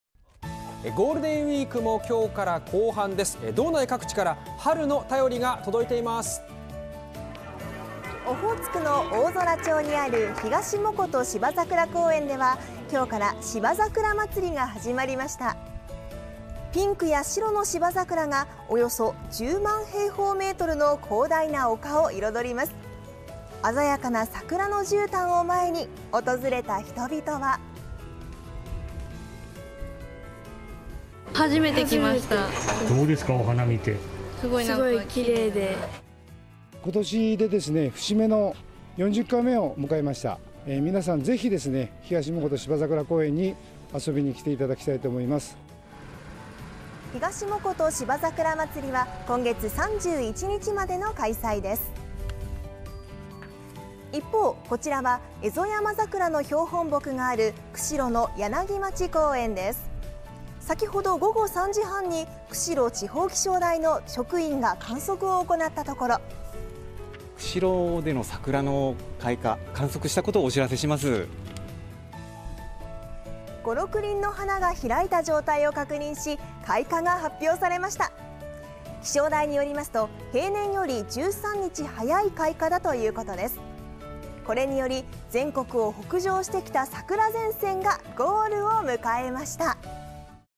列島を北上してきた「桜前線」が釧路市でゴール…気象台の標本木エゾヤマザクラが開花 大空町では芝桜が広大な丘を彩る
ゴールデンウイークも３日から後半。北海道各地から春の便りが届いています。

　オホーツクの大空町にある東藻琴芝桜公園では、３日から「芝桜まつり」が始まりました。
　ピンクや白の芝桜がおよそ10万平方メートルの広大な丘を彩ります。
　鮮やかな「桜のじゅうたん」を前に訪れた人々は…。

来園者
「初めて来ました。すごくきれい」

東藻琴芝桜公園管理公社　田中信裕総務課長
「今年で節目の40回目を迎えました。皆さんぜひ東藻琴芝桜公園に遊びに来てほしいと思います」

　ひがしもこと芝桜まつりは、31日までの開催です。

　一方、こちらはエゾヤマザクラの標本木がある釧路市の柳町公園です。
　３日午後３時半に、釧路地方気象台の職員が観測を行ったところ…。

気象台職員
「開花です」

　５～６輪の花が開いた状態を確認し、開花が発表されました。
　気象台によりますと、平年より13日早い開花だということです。
　これにより全国を北上してきた「桜前線」がゴールを迎えました。2024年05月03日(金) 17時48分 更新